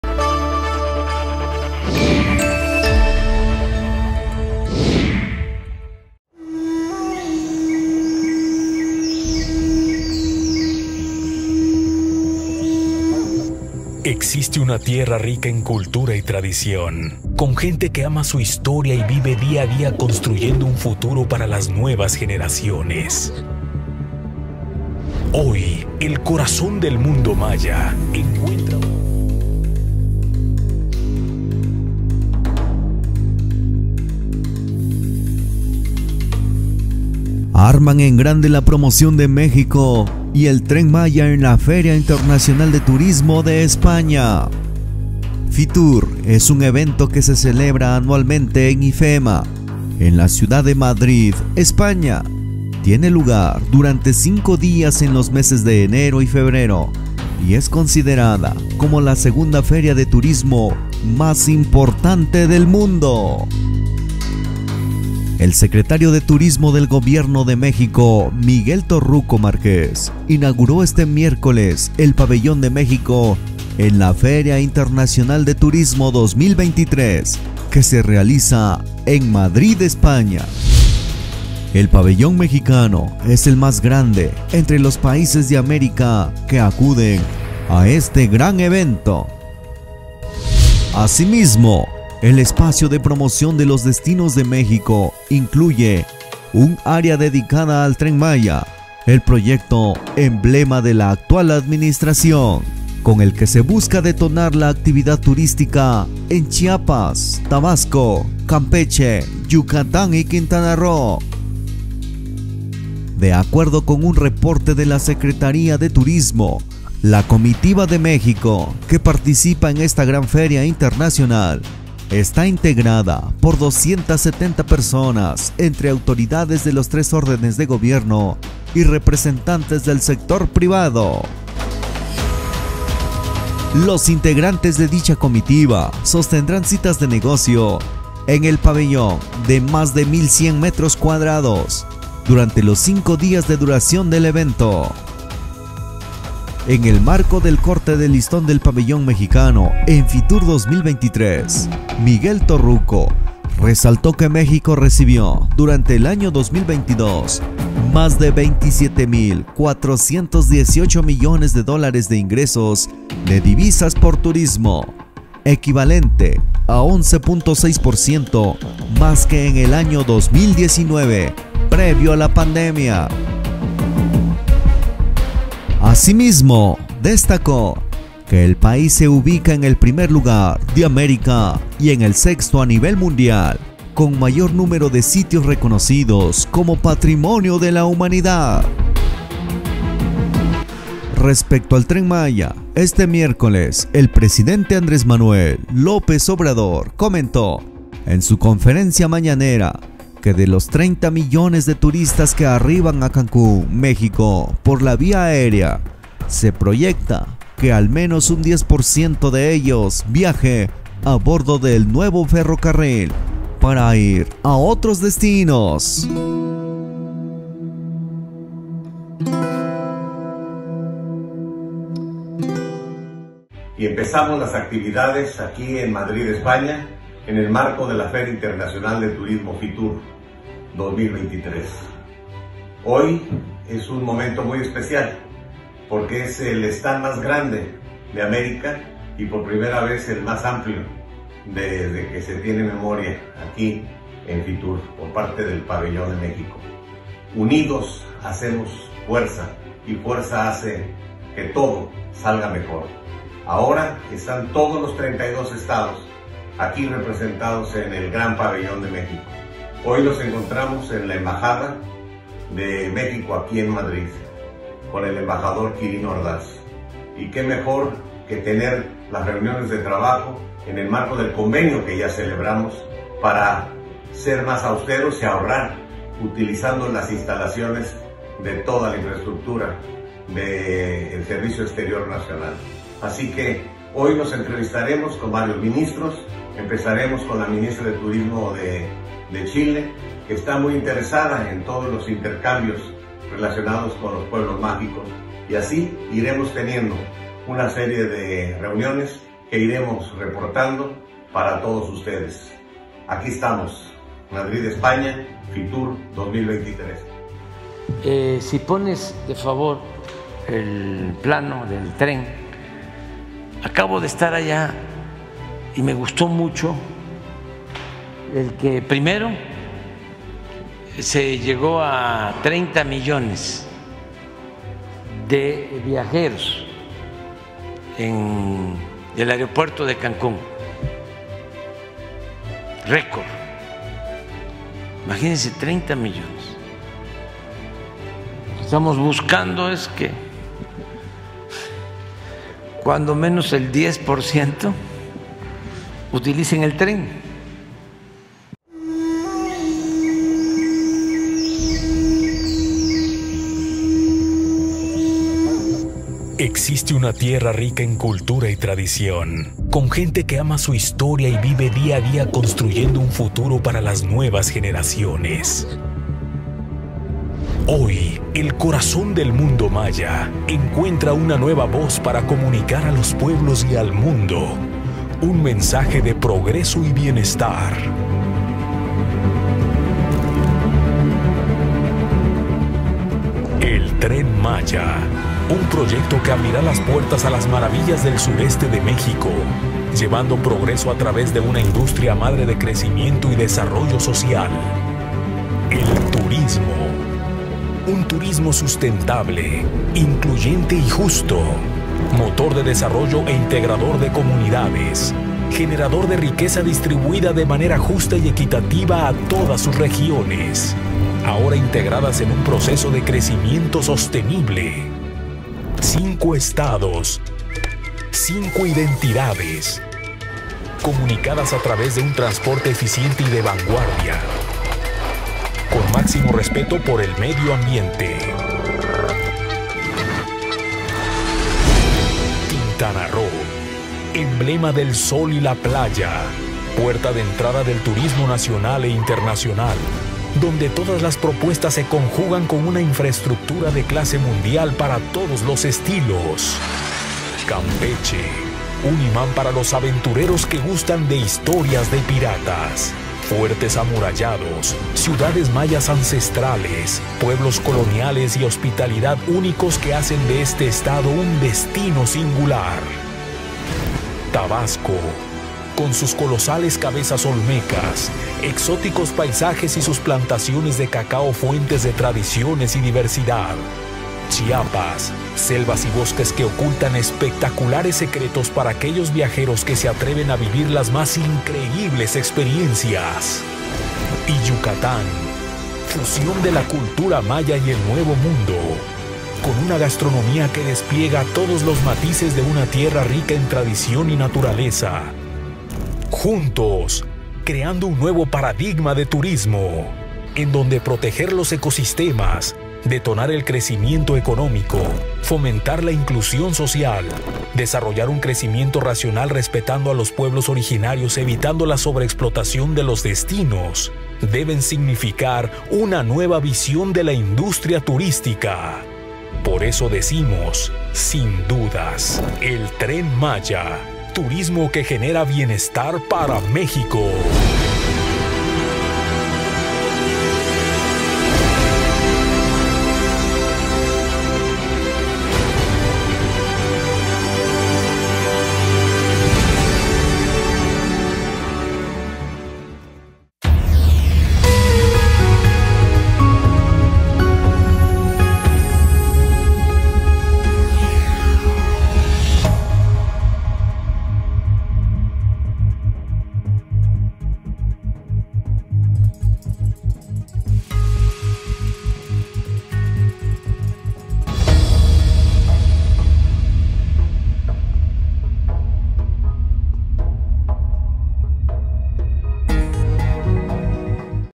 Ante los ojos de todo el Mundo presenta el Proyecto del Tren Maya desde Madrid, España.
Impresionante el gran proyecto del Tren Maya, inicia su promocion en Madrid,España